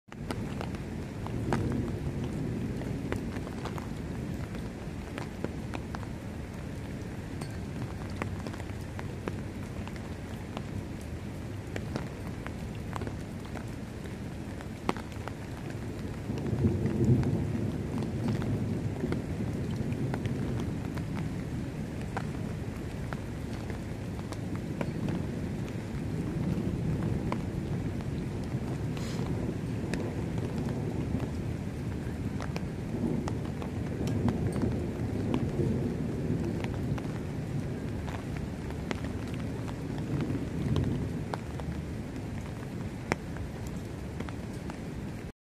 Lightning striking every few seconds during a thunderstorm
Zeus is on a riot tonight.

Original title:
VID_20240430_221047.mp4